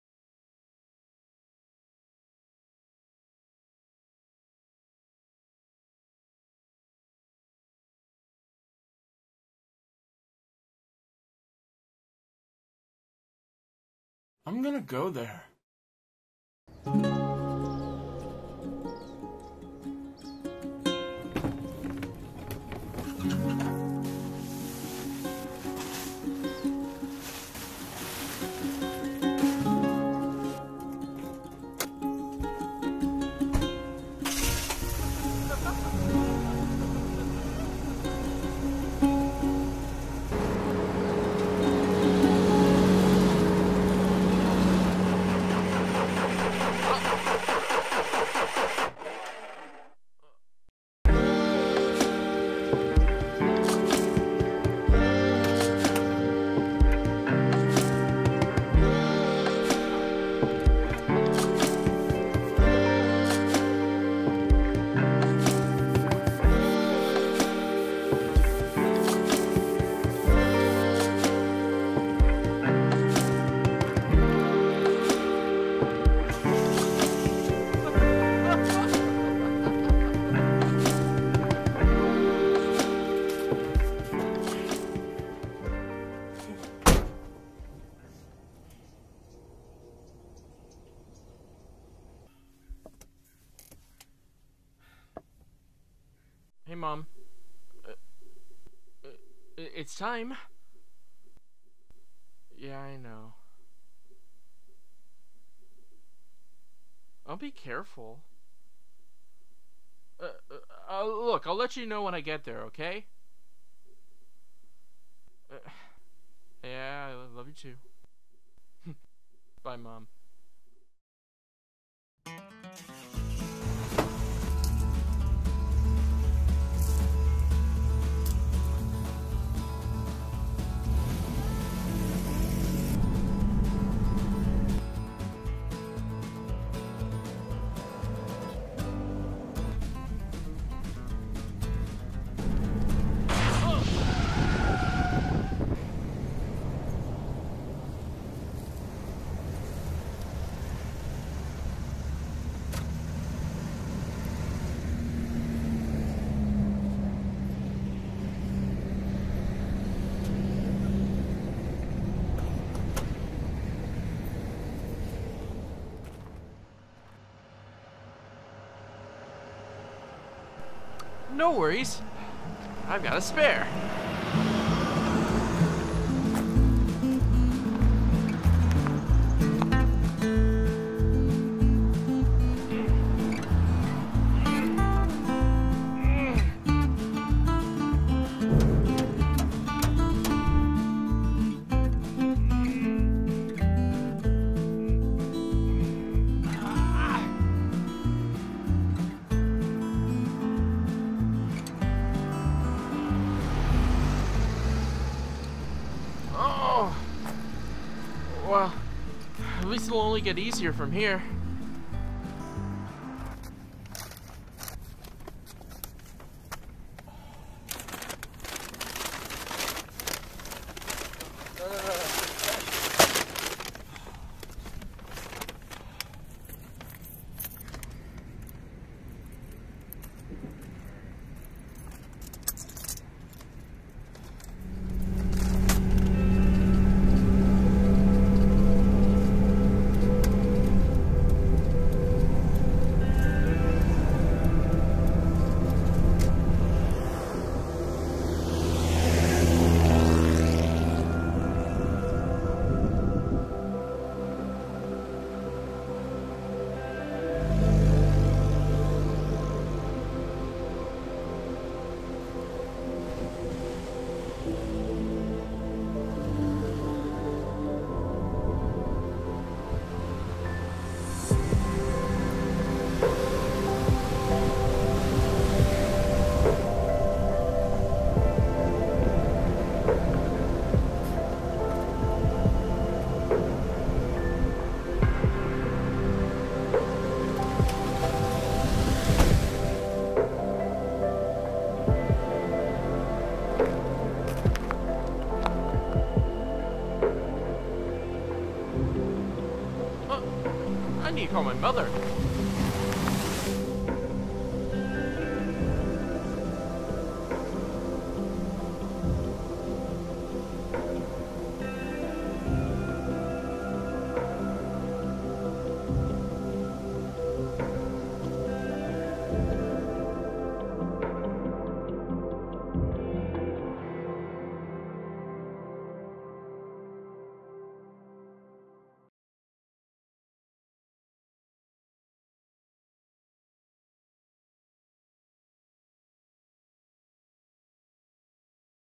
At the Edge of the Earth- Kyle Burkhart Video I Final
In glorious 4K.
My short film I created for my video class, from treatment to completion.